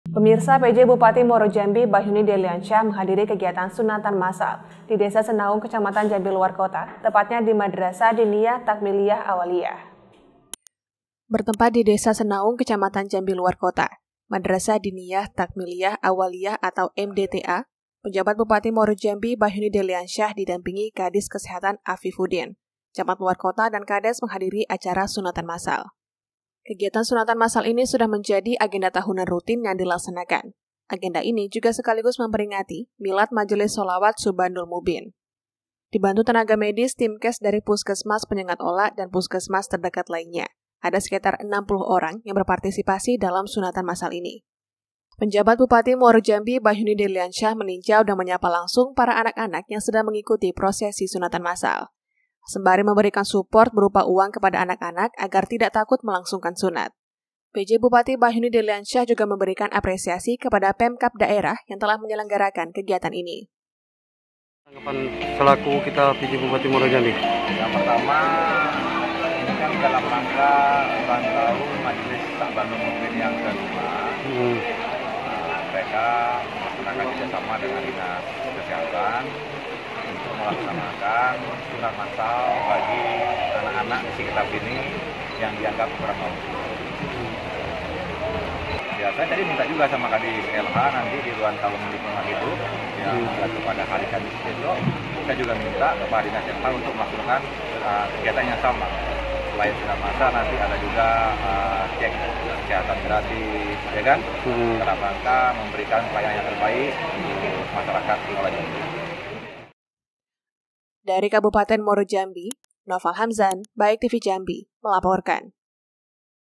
PJ BUPATI BACHYUNI DELIANSYAH MENGHADIRI SUNATAN MASSAL DI SENAUNG
BNEWS EDISI 23 JUNI 2023